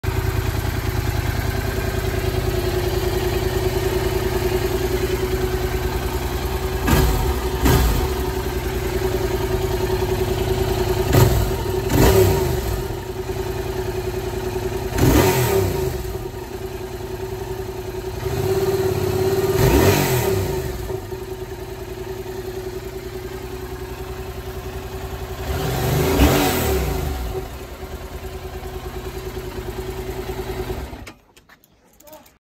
Yamaha MT07 Stock Exhaust RAW 4K
MT07 Stock Exhaust sound RAW

Quick Sound Check

Helmet - Shoei XR1100

Gloves - Fox Racing Bomber Gloves

Bike - Yamaha MT07 Motocage